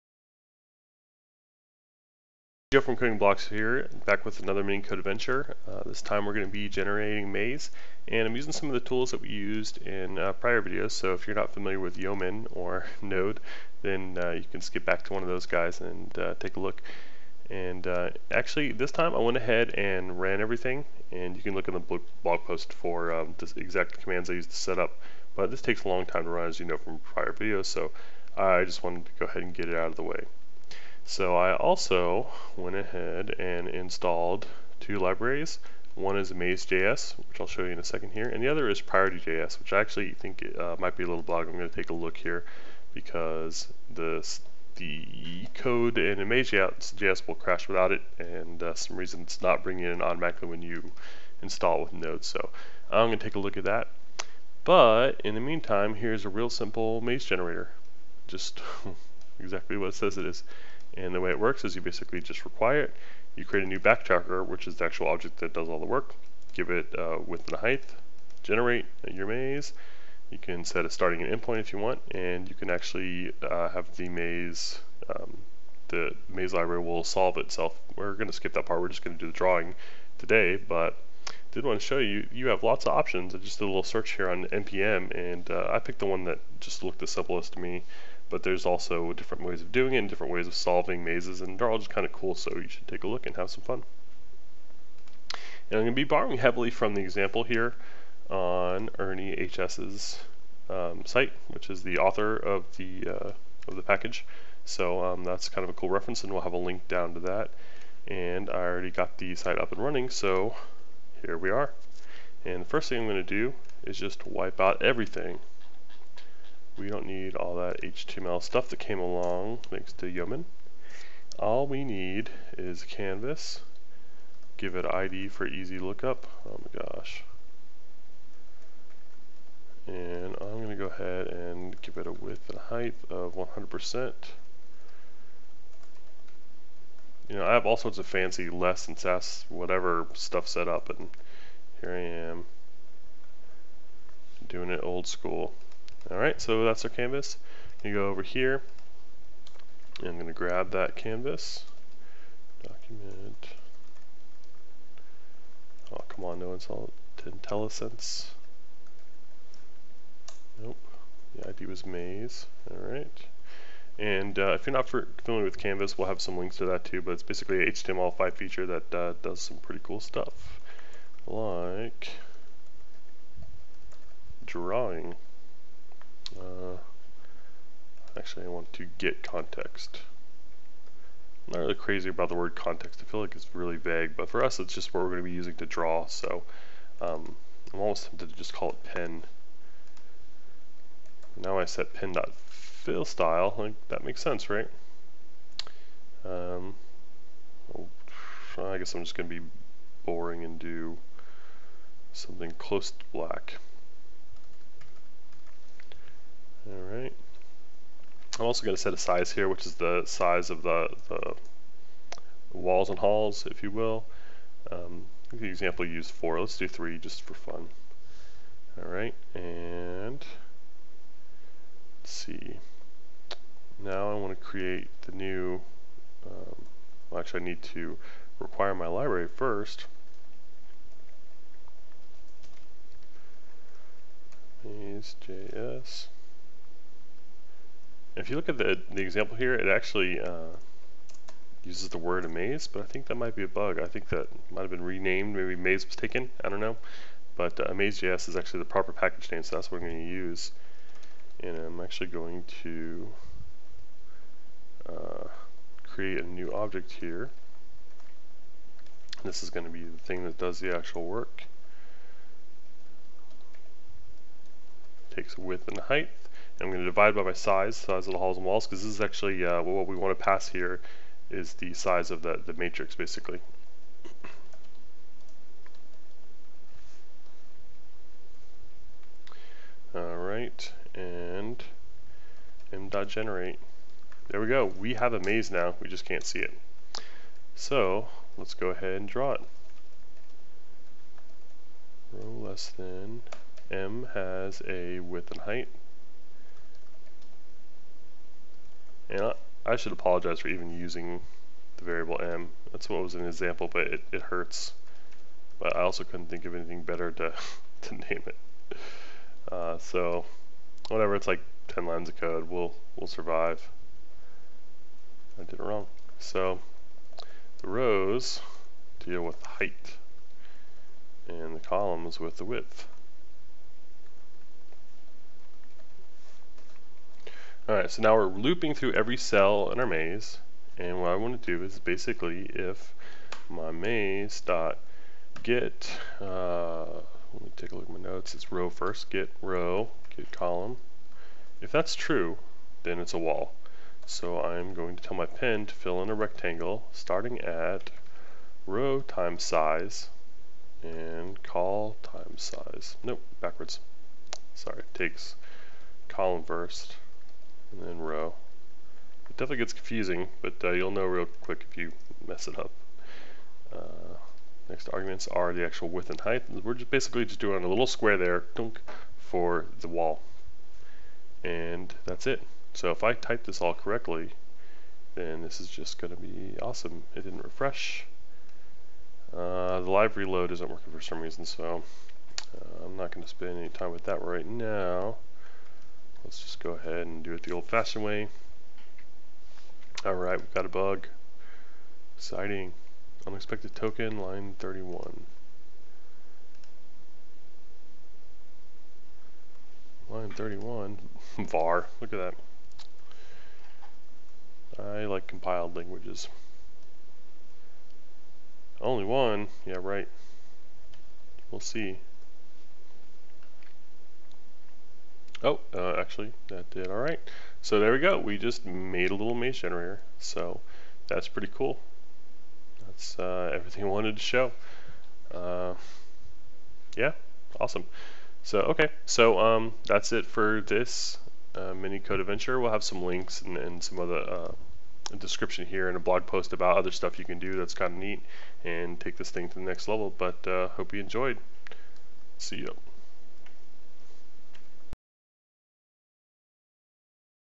Mini Code Adventures: Generating Mazes in JavaScript
Generating mazes in JavaScript is fun and easy. Follow along as we set up a simple website to generate perfect mazes.